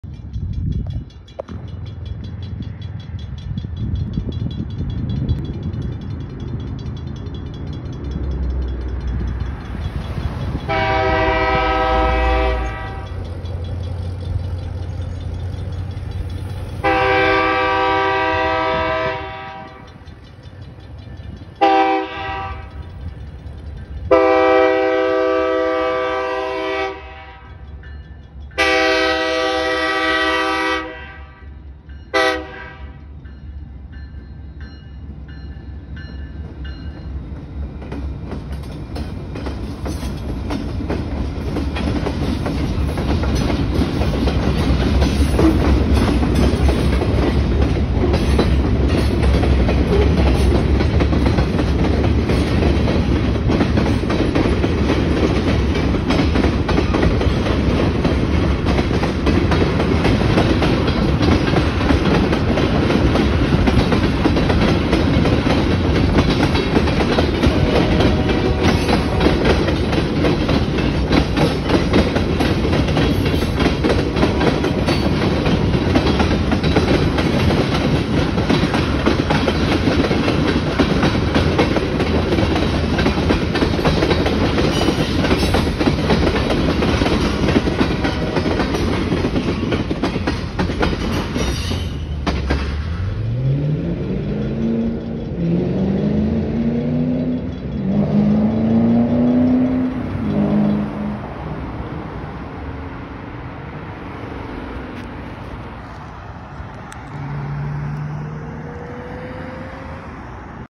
BNSF 145 leads the second local back towards Watson Yard after passing Sepulveda Blvd
Taken just after 11:10pm with 145 leading and 142 trailing and I got a video of them as they were heading towards Watson Yard in Wilmington from the Alcoa Yard in Torrance and the train is carrying two hopper cars and lots of oil tanker cars that are all heading towards Watson Yard and I was able to get it after hearing 145s horn and thanks to the crew of 145 for waving at me and enjoy the video people